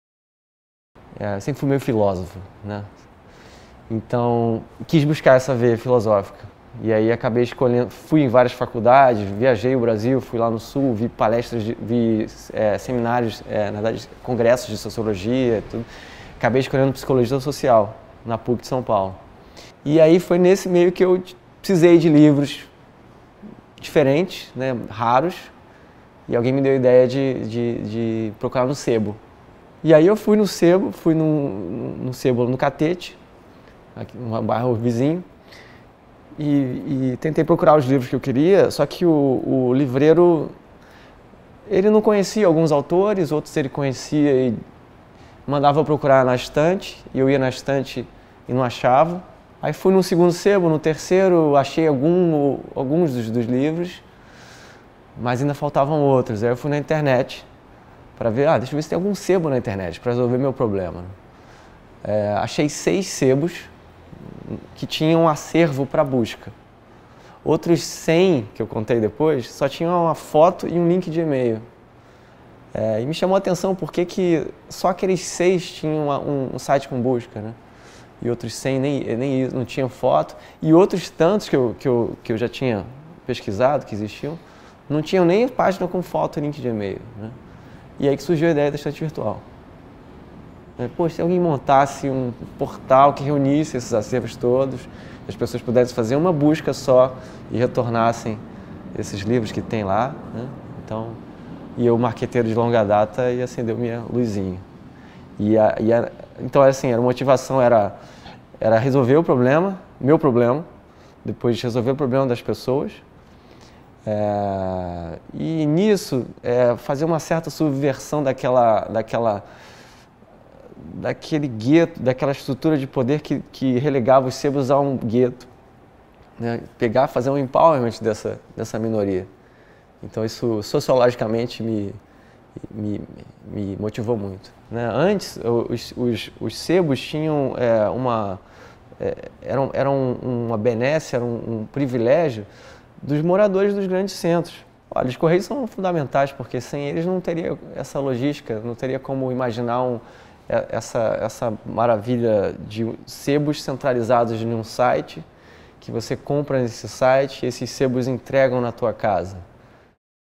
Sebos não são mais guetos - Estante Virtual
André Pereira Garcia Ramos, filho de um engenheiro e uma professora, relata em sua história de vida que durante sua infância adorava montar e pintar aviões de plástico, nadar, soltar pipa e pescar.  Desenvolveu o gosto pela leitura apenas entre seus 18, 19, anos com o livro “O Buraco na Parede” de Rubens Fonseca. Formou-se em Administração de Empresas e concluiu um mestrado em Psicologia Social. No período de preparação para o mestrado André precisou ler muitos livros e encontrou diversas dificuldades para pesquisar acervos de sebos, assim nasce a ideia da Estante Virtual, montar um portal reunindo esses acervos para que pessoas possam fazer apenas uma única busca. Lançada em 2005 a Estante Virtual é o portal de comércio eletrônico que reúne o maior acervo de sebos e livreiros do Brasil e conta com a parceria dos Correios para democratizar o acesso de todo o Brasil a tais acervos.